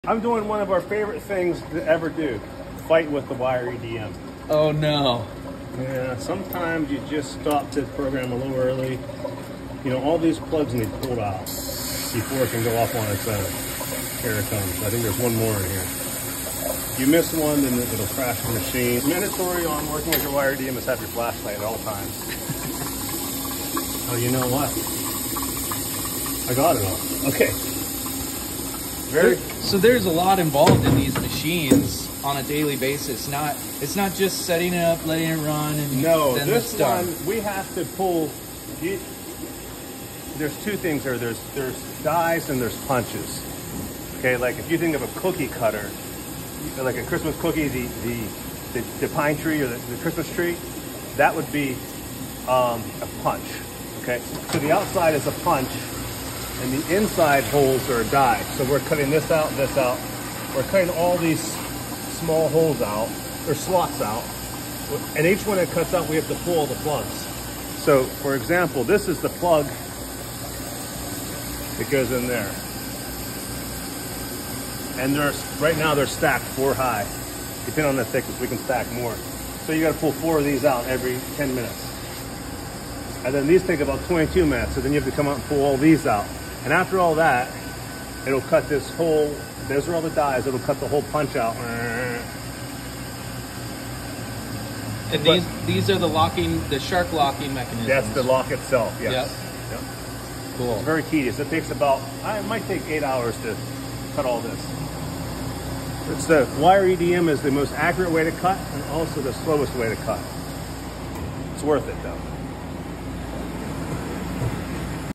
Inside Look - The wired EDM maching cutting shark-locks
Andrew gives us an inside look at the wire EDM and the process of cutting out locks.